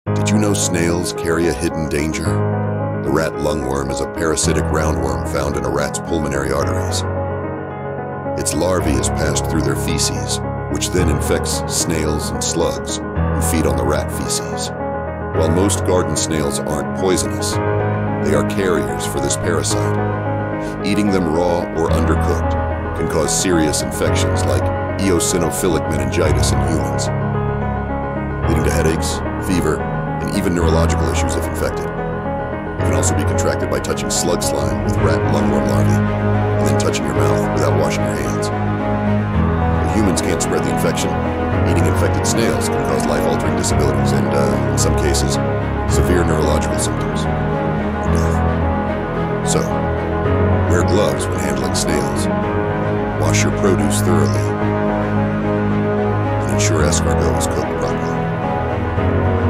Are Snails Poisonous? Facts Revealed!
Did you know that some snails and slugs in your garden could carry a dangerous parasite?
The rat lungworm, scientifically called Angiostrongylus cantonensis, is a parasitic roundworm that completes its life cycle in rats but can accidentally infect humans through snails, slugs, and even their slime. While snails themselves aren’t poisonous, this parasite poses serious health risks.
Life Cycle of the Rat Lungworm
The parasite’s life begins in rats, where adult worms live in the pulmonary arteries. Larvae pass through rat feces, which snails and slugs ingest, becoming carriers. When humans accidentally ingest contaminated snails, slugs, or produce, the parasite enters the human body. While it can’t fully mature in humans, the larvae can migrate to the brain and spinal cord, causing eosinophilic meningitis.
Dangers to Humans
Ingesting the rat lungworm can lead to eosinophilic meningitis, a condition that inflames the brain and spinal cord, causing severe symptoms:
1. Headaches and Fever: Early symptoms mimic the flu, often leading to a delay in diagnosis.
2. Neurological Issues: Nerve damage can result in numbness, tingling, or paralysis.
3. Severe Cases: In rare cases, severe infection can lead to life-altering disabilities or even death, though the latter is uncommon.
The parasite is most commonly spread when humans eat raw or undercooked snails, slugs, or contaminated produce. Here’s how the infection can occur:
• Eating Raw Snails or Slugs: Accidentally ingesting a snail or slug can lead to infection.
• Handling Contaminated Snails: Touching infected snails or their slime and then touching your mouth can transfer larvae.
• Contaminated Produce: Fresh produce, especially leafy greens, can harbor snail or slug slime.
How to Stay Safe
1. Cook Thoroughly: If you eat snails, ensure they are fully cooked to kill the parasite larvae.
2. Wear Gloves: When gardening or handling snails, wear gloves to avoid direct contact with slime.
3. Wash Hands: Always wash your hands thoroughly after handling snails, slugs, or working in the garden.
4. Clean Produce: Thoroughly wash all fresh produce to remove potential slime trails.
5. Be Cautious in Endemic Areas: In regions where rat lungworm is common (Southeast Asia, the Pacific Islands, and parts of the U.S.), take extra precautions.
Ecological Role of Snails
Despite the risks to humans, snails play a crucial role in the ecosystem. They help decompose organic matter and serve as a food source for animals like birds and frogs. So, while it’s important to handle them safely, they contribute to the health of our environment.
Conclusion
While snails and slugs are an important part of nature, they can carry the rat lungworm parasite, leading to severe infections if ingested. By following simple precautions, like cooking snails thoroughly, wearing gloves, and washing produce, you can stay safe from this hidden danger. Always be aware of the risks and protect yourself when handling or consuming snails.